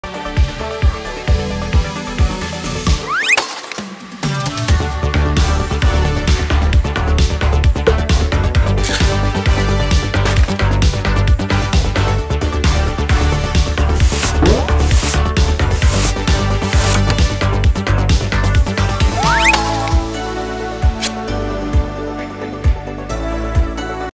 Good deeds are sure to be rewarded || Good VS Bad Person || MOBILE GAME ADS BE LIKE
Why Should You Do Acts of Kindness?